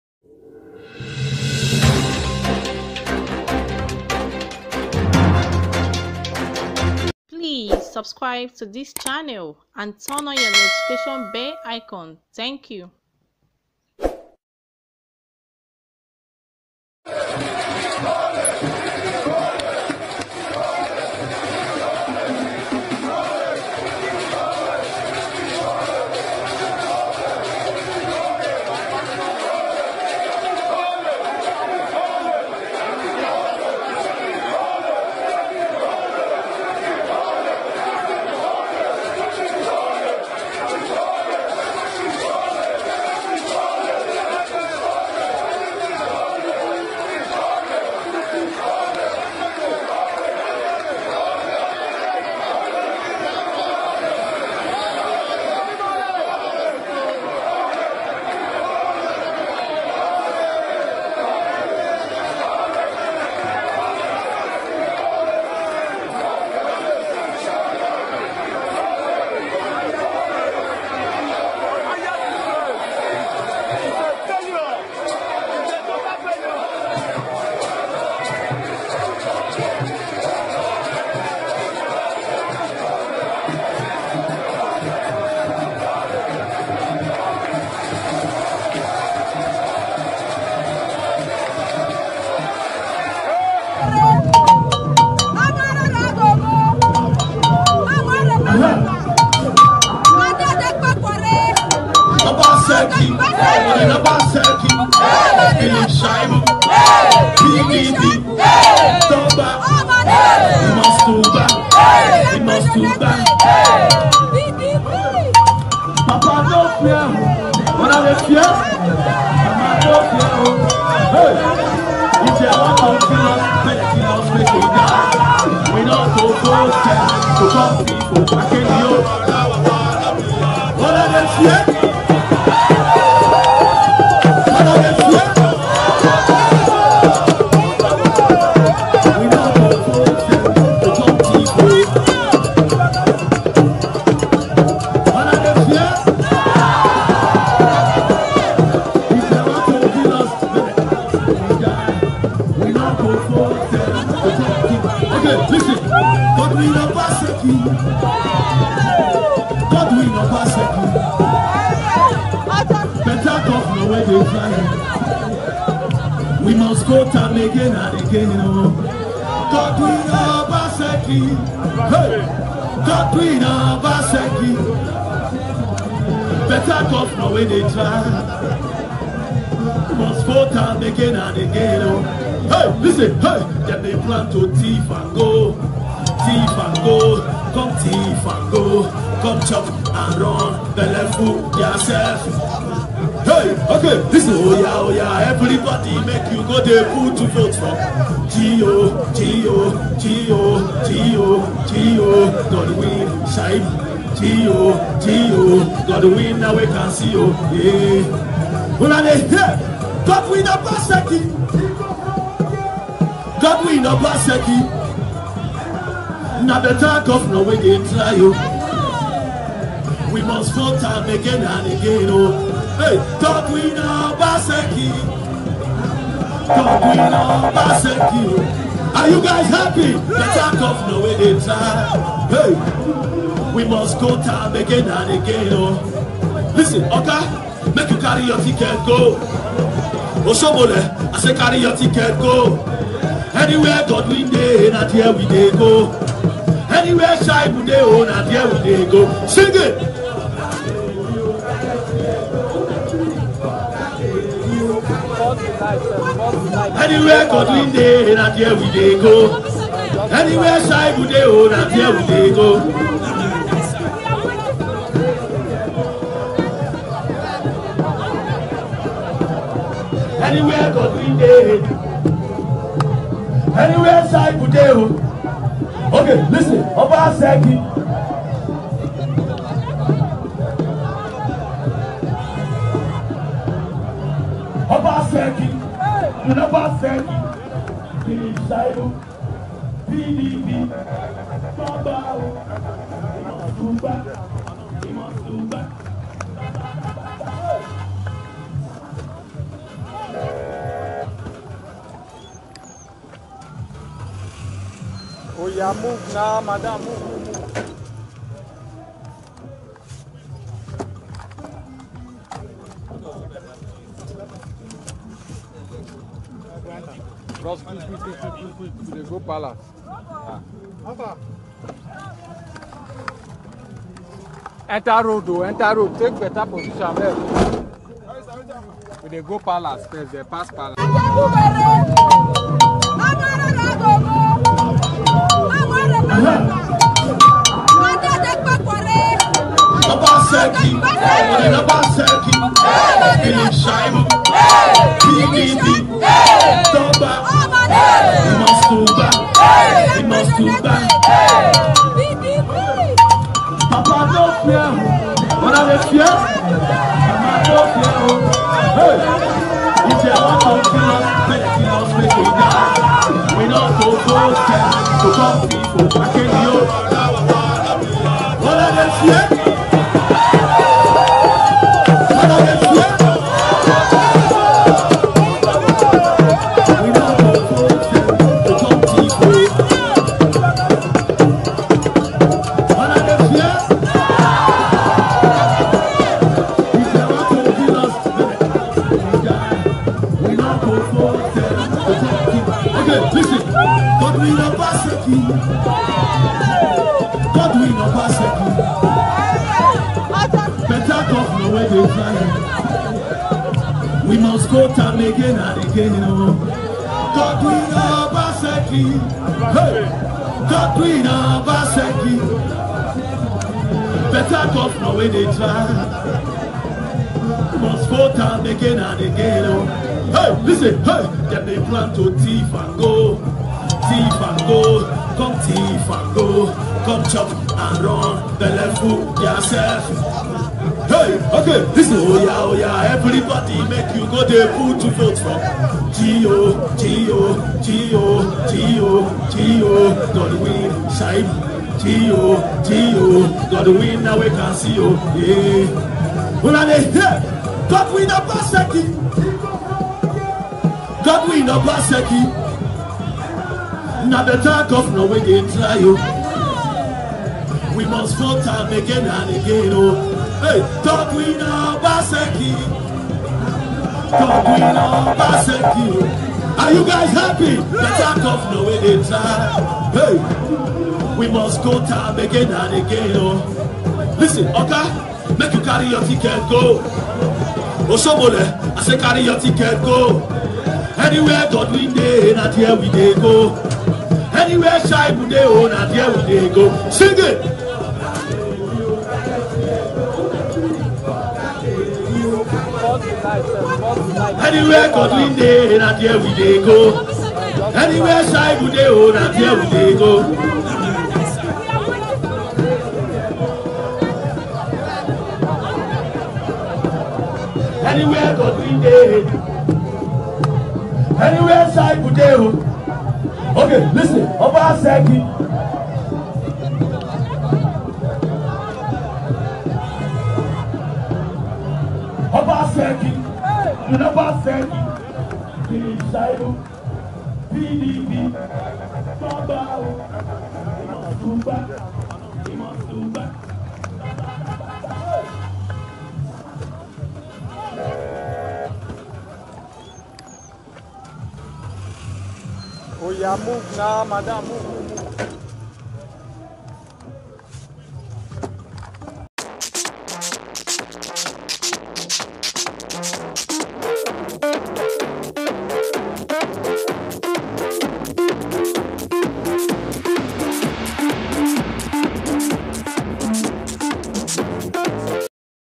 EDO 2020: OBASEKI'S CAMPAIGN TRAIN ATTACKED, EDO BLAMES APC THUGS
Naija news HD is the place where you get Trending and Latest Nigeria news.

Do well to turn on your Notification 🔔 Icon